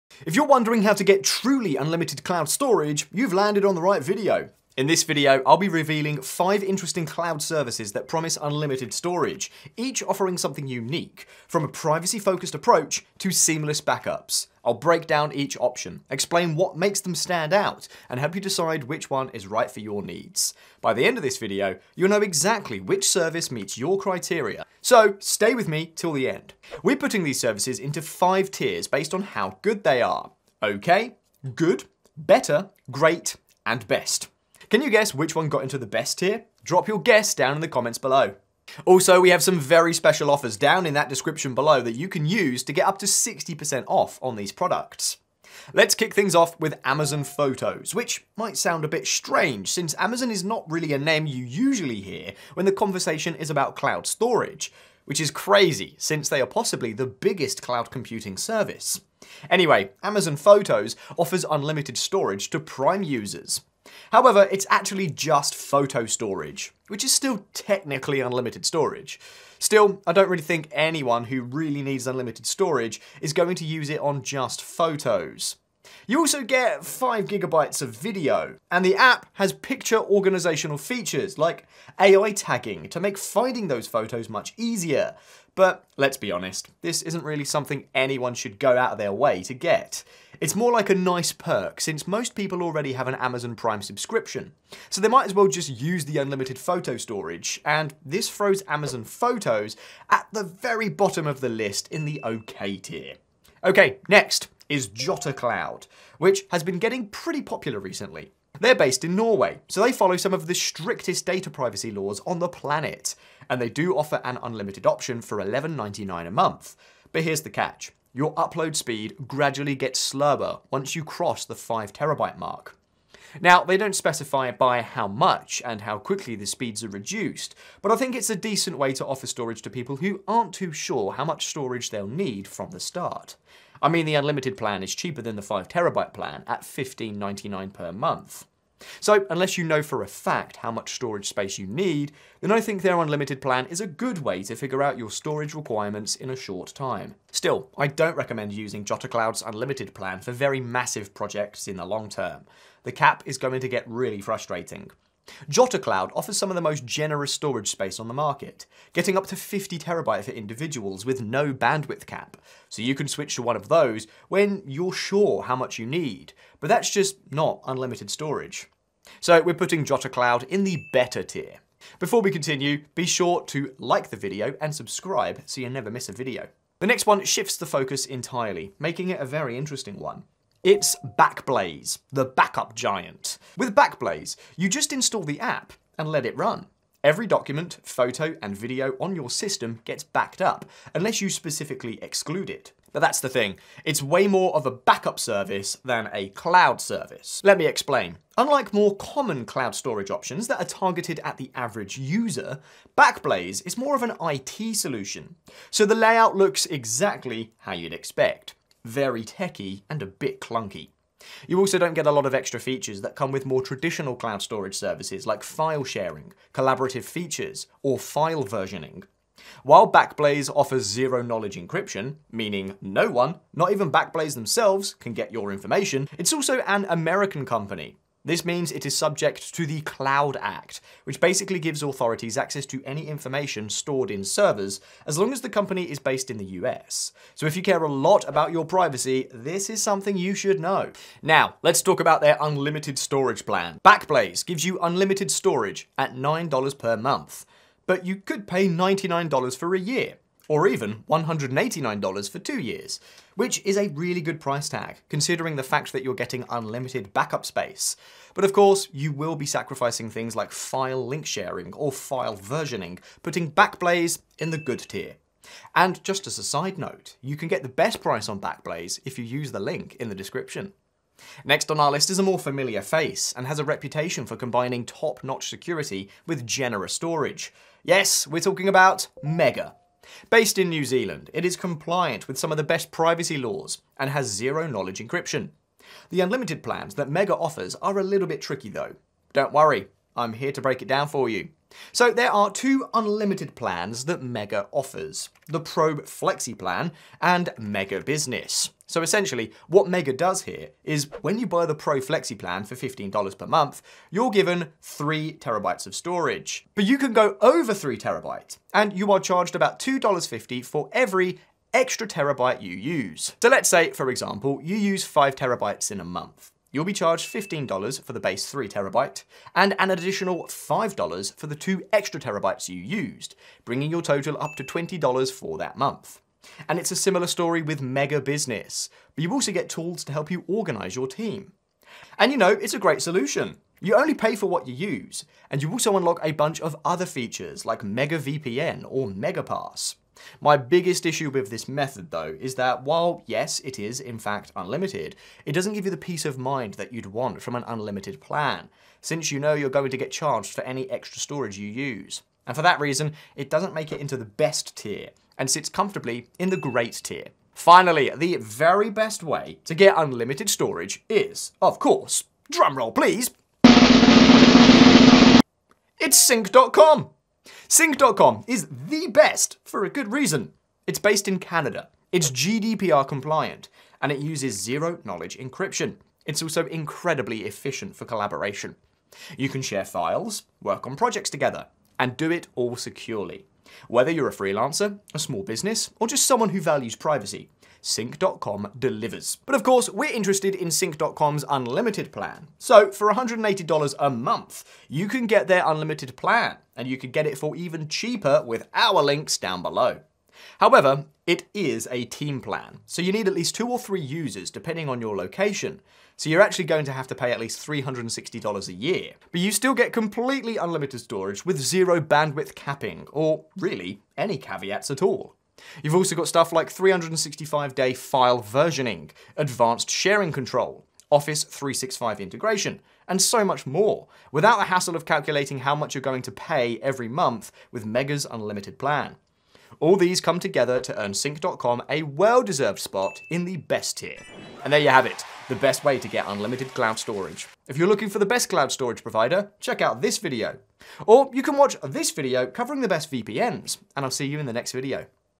Get Unlimited Cloud Storage (100% Legit)
The best unlimited cloud storage providers 👇 (official links)
👉 MEGA (no link)

If you’re wondering how to get truly unlimited cloud storage, you have landed on the right video.

In this video, I’ll be revealing five interesting cloud services that promise unlimited storage, each offering something unique, from a privacy-focused approach to seamless backups.

I’ll break down each option, explain what makes them stand out, and help you decide which one is right for your needs. By the end of this video, you’ll know exactly which service meets your criteria, so stay with me till the end.

We will be putting these services into five tiers based on how good they are: Okay, Good, Better, Great, and Best.

Can you guess which one got into the Best tier? Drop your guess in the comments below.

📜 Video Chapters

0:00 Intro
0:45 Amazon Photos
1:43 Jottacloud
2:56 Backblaze
4:30 MEGA
5:57 Sync